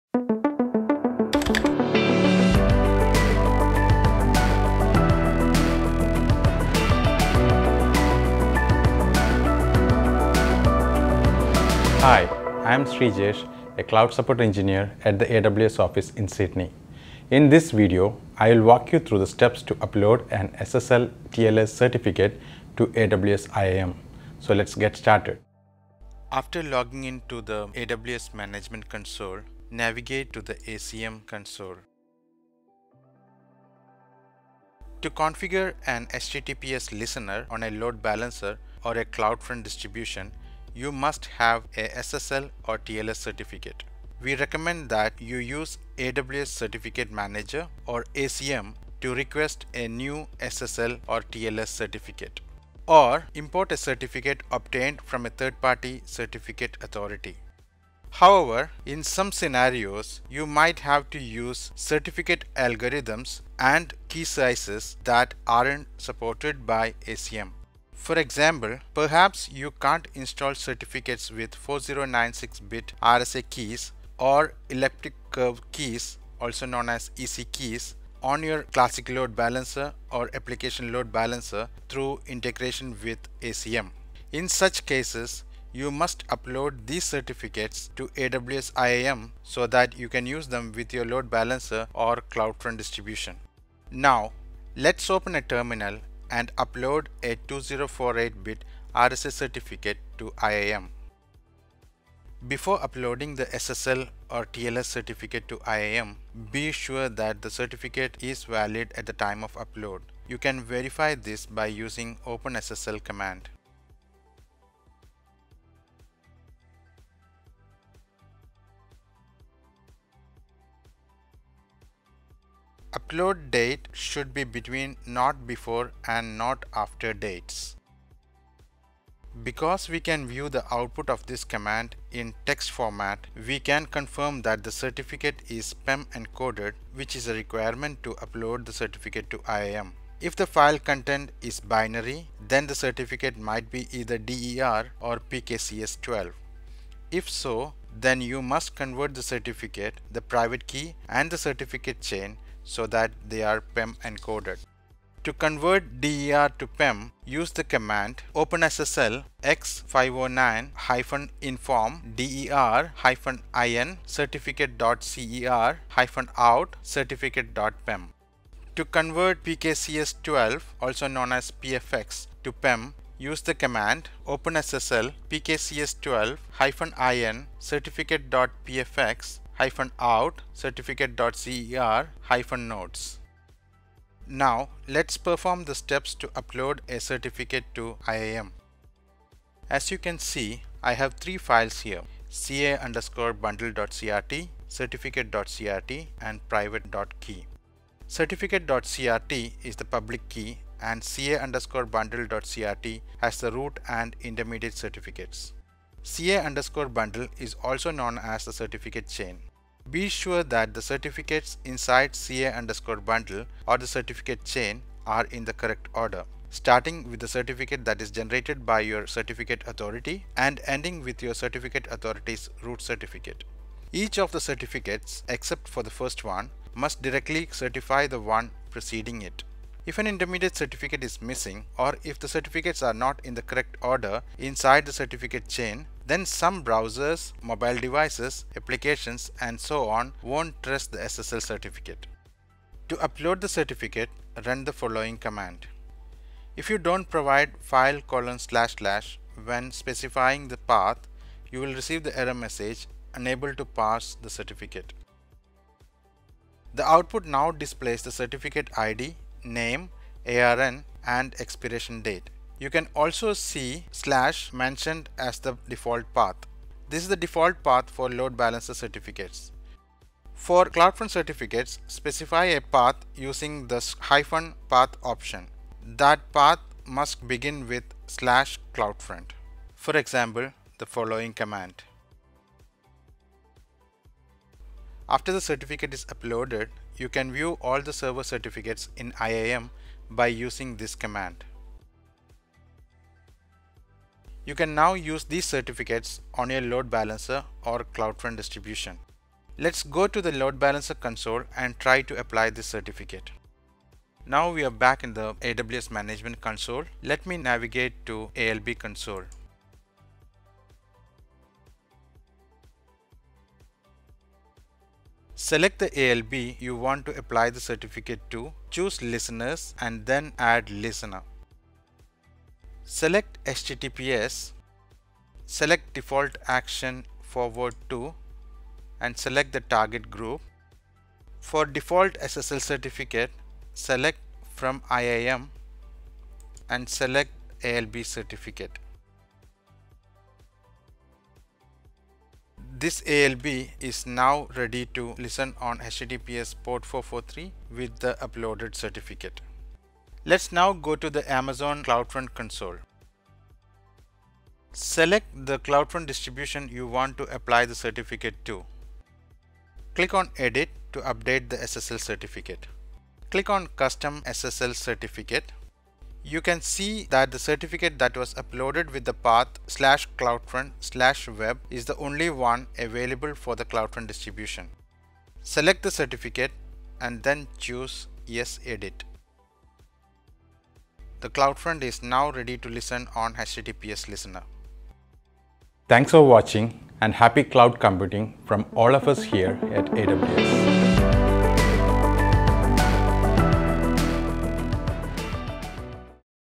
How can I upload and import an SSL certifcate to AWS IAM?
Shree, an AWS Cloud Support Engineer, shows you how to upload and import an SSL certificate to AWS Identity and Access Management (IAM)?